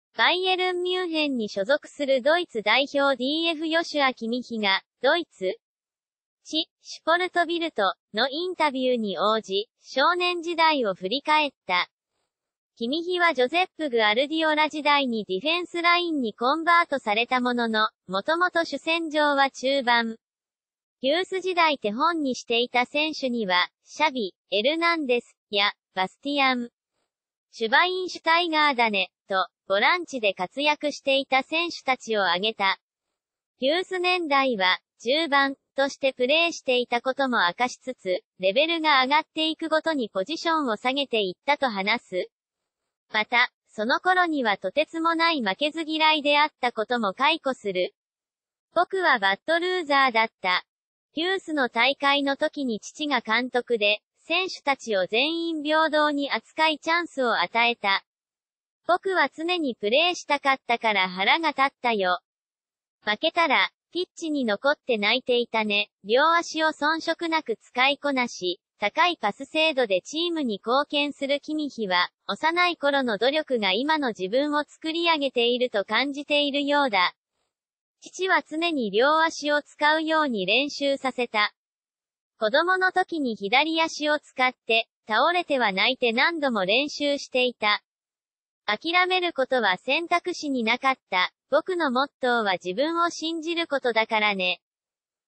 ドイツ代表dfキミヒの知られざる少年時代「僕はバッドルーザーだった」-サッカー ニュース
バイエルン・ミュンヘンに所属するドイツ代表DFヨシュア・キミヒが、ドイツ誌『シュポルト・ビルト』のインタビューに応じ、少年時代を振り返った。 キミヒはジョゼップ・グアルディオラ時代にディフェンスラインにコンバートされたものの、元々主戦場は中盤。ユース時代手本にしていた選手には「シャビ（エルナンデス）や（バスティアン）シュバインシュタイガーだね」と、ボランチで活躍していた選手たちを挙げた。 ユース年...